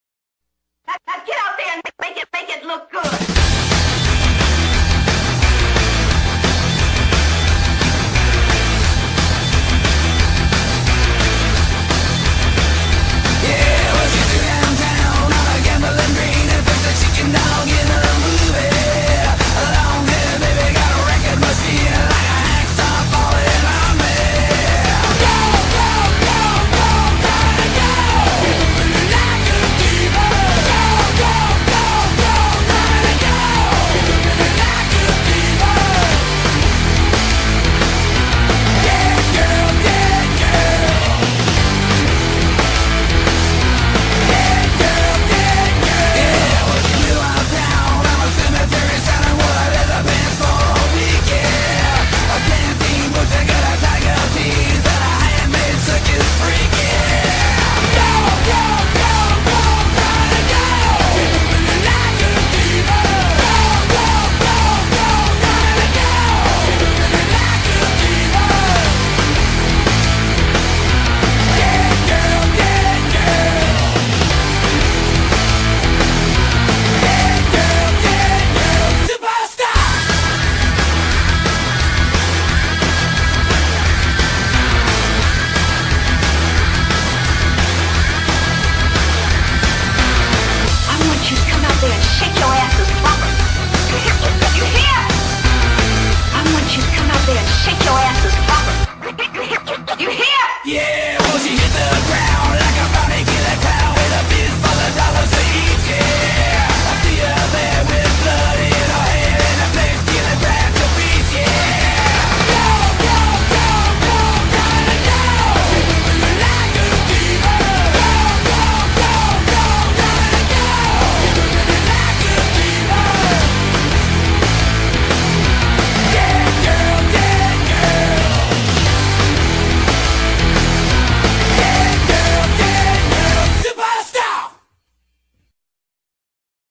Rob Zombie School Girl Sword Dance
My wolf cane is my right hand sword. I'm teaching myself how to wield it.